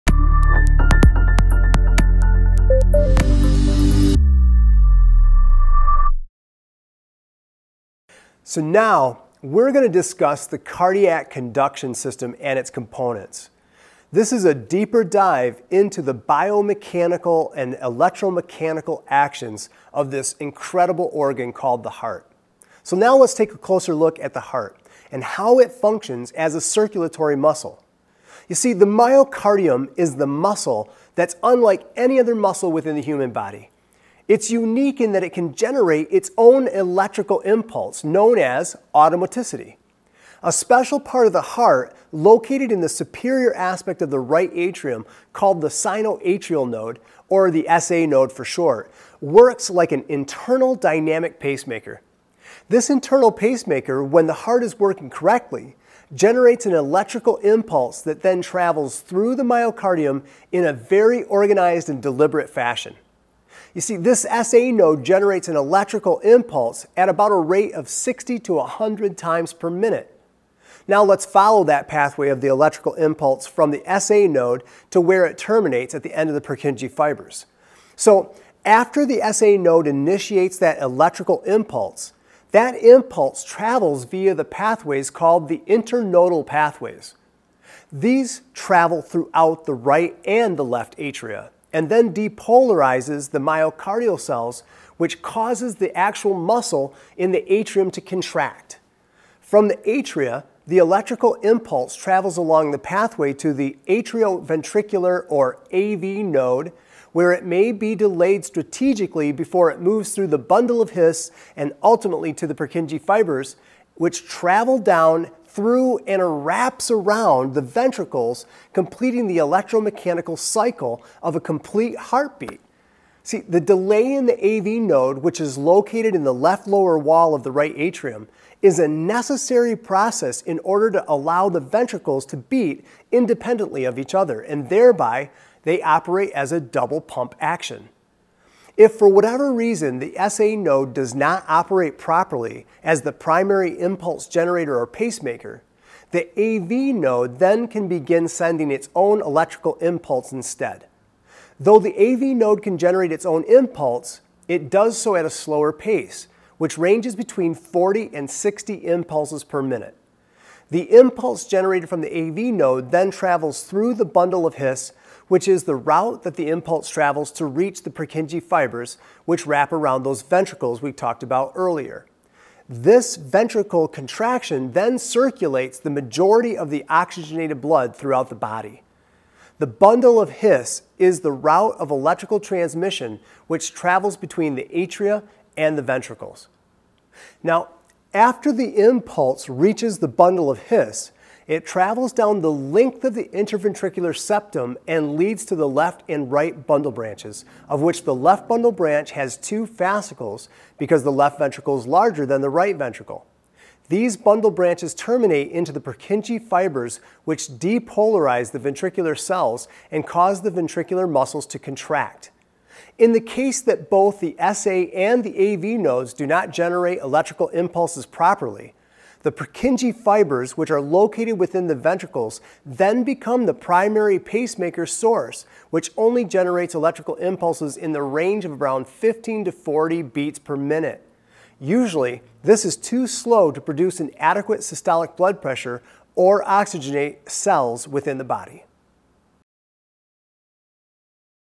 The Cardiac Conduction System | ACLS & PALS Training Video
The myocardium, or heart, is a muscle unlike any other muscle within the human body. It’s unique in that it can generate its own electrical impulse known as automaticity. In this video we will discuss the pathway of the electrical impulse from the SA node in the atria, through the middle of the heart and to where it terminates at the end of the Purkinje fibers in the ventricles.